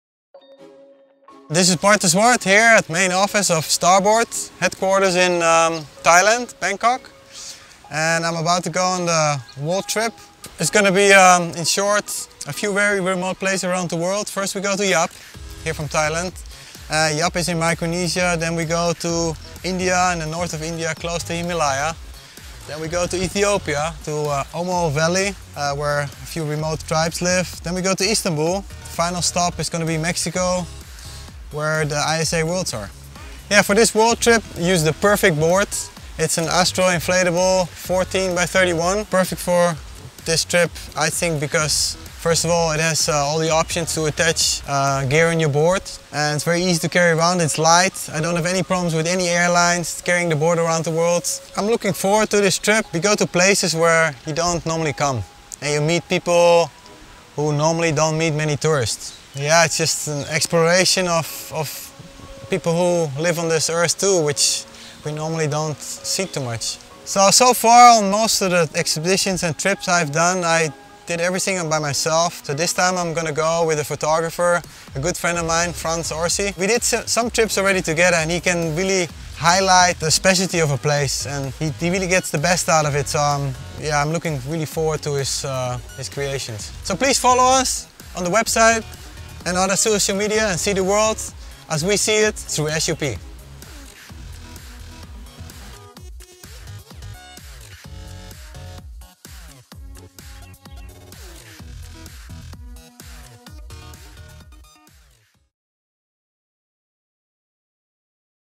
Bart De Zwart World Tour Promo
Starboard team rider Bart de Zwart along with photographer Franz Orsi will embark on a world wide tour to explore uncharted locations via Stand Up Paddle Boards.

They aim to capture as much as possible through photography and video.

So follow them on the following social media platforms.